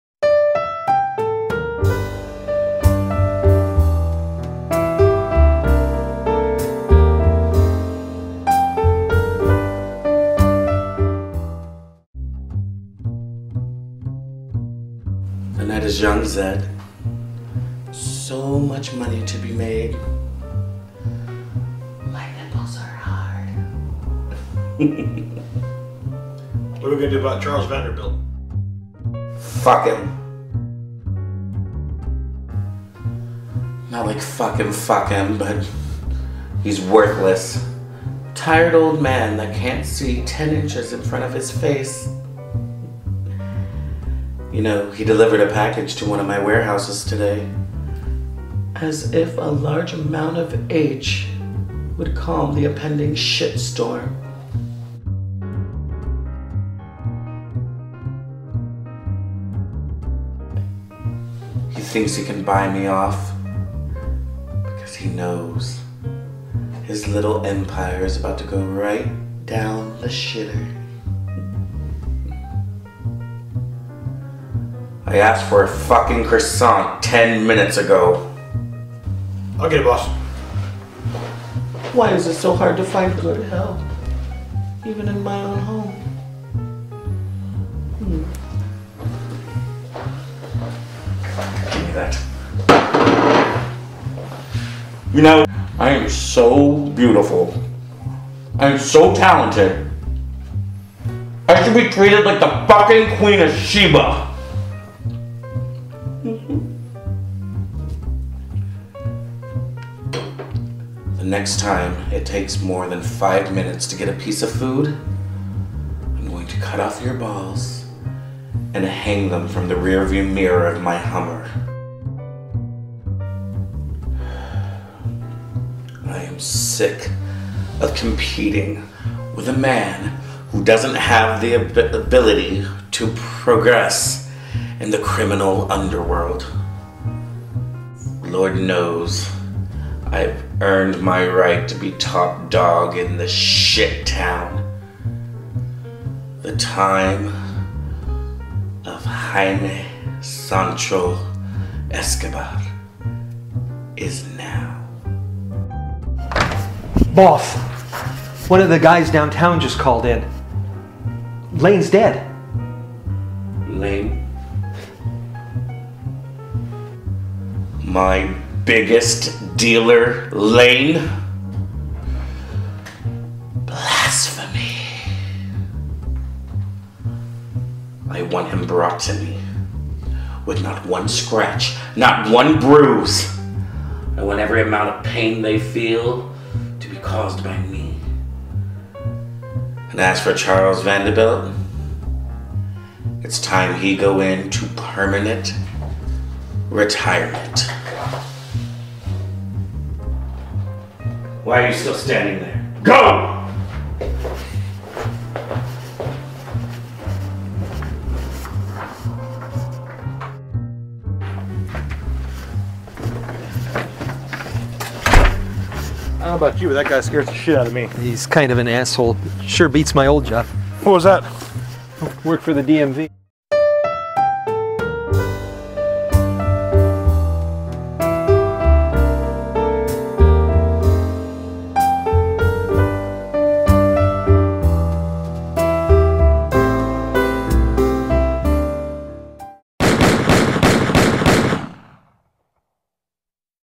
Two Bit Killers: "Introducing Jaime Sancho Escobar"
In this episode, Jaime makes a decision about how to deal with his rival, Charles Vanderbilt. Jaime is also presented with bad news concerning one of his men.